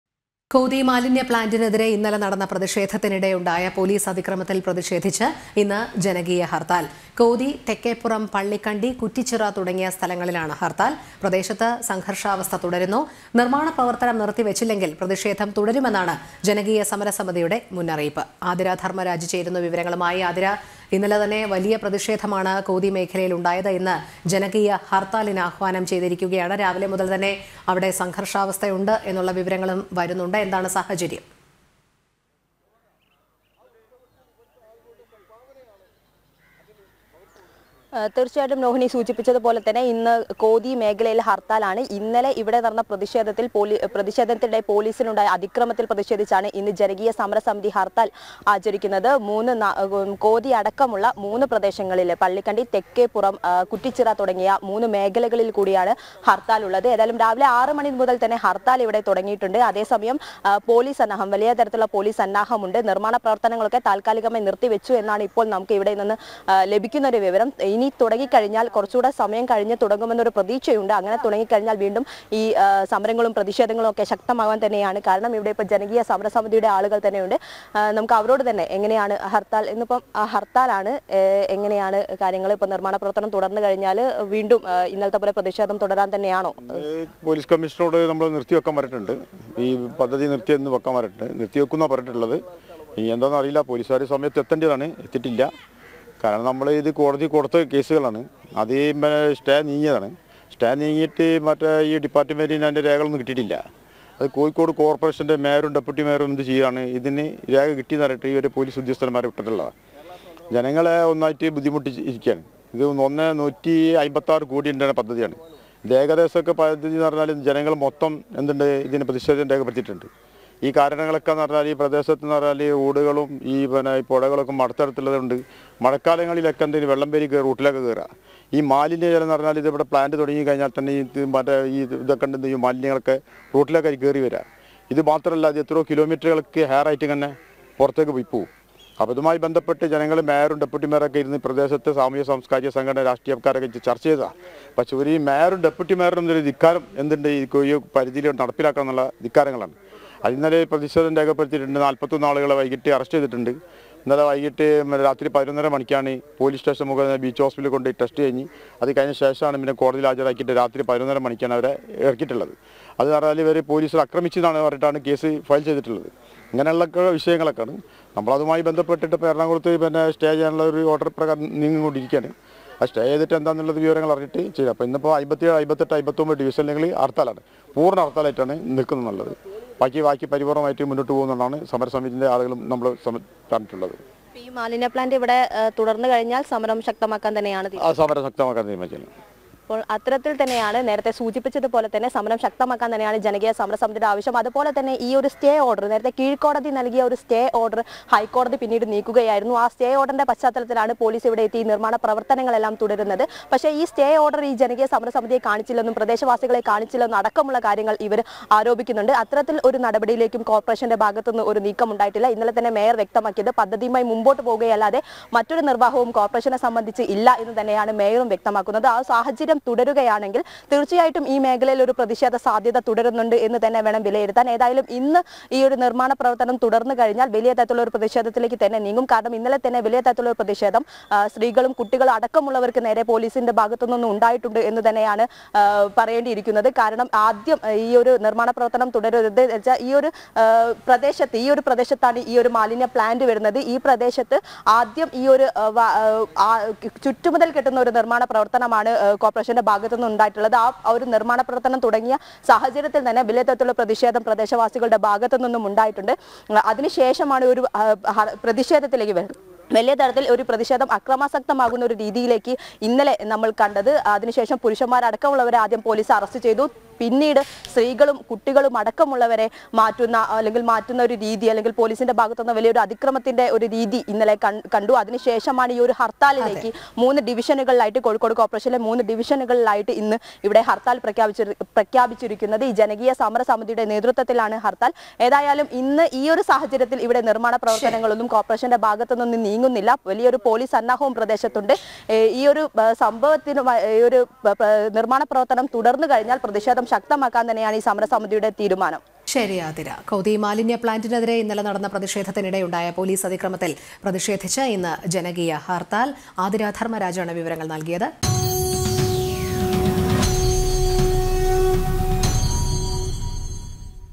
കോതിയിൽ ഇന്ന് ജനകീയ ഹർത്താൽ | JANAM TV
കോതി മാലിന്യ പ്ലാന്‍റിനെതിരെ ഇന്നലെ നടന്ന പ്രതിഷേധത്തിനിടെ ഉണ്ടായ പോലീസ് അതിക്രമത്തിൽ പ്രതിഷേധിച്ച് ഇന്ന് ജനകീയ ഹർത്താൽ

NEWS ANCHOR : ROHINI MAHESH, KEERTHANA T